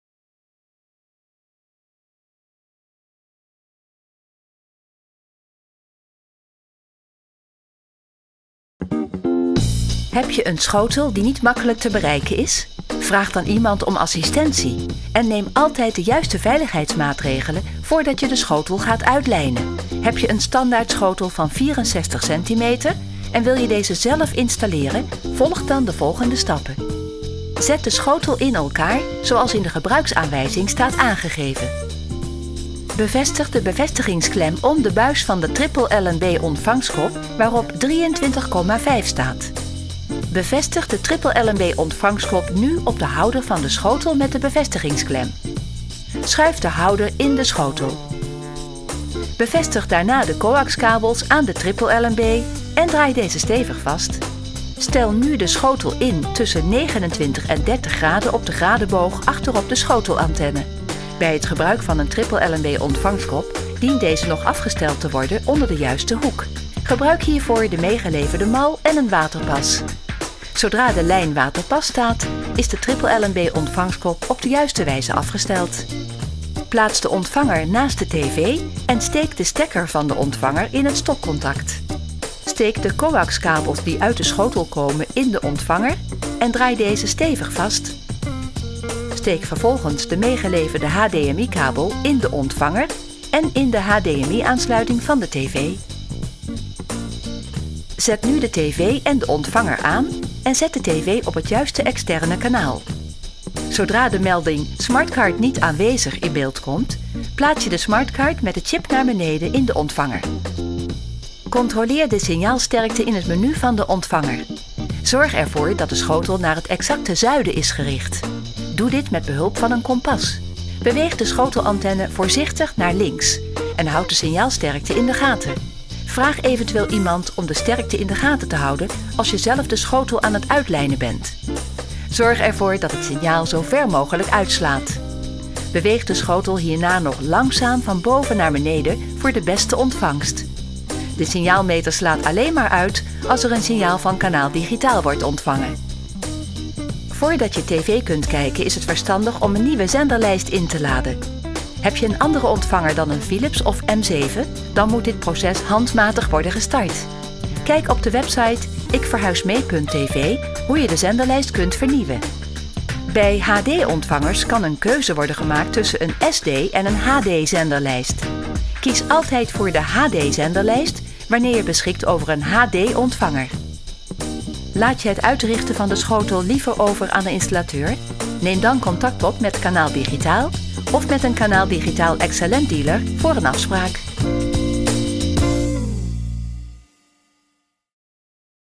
TripleSat met TripleLNB van CanalDigitaal installeren - klantenservice
Heb je een nieuwe TripleSat-schotelantenne, met een TripleLNB gekocht? Kijk dan naar dit filmpje en je weet meteen hoe je hem moet installeren.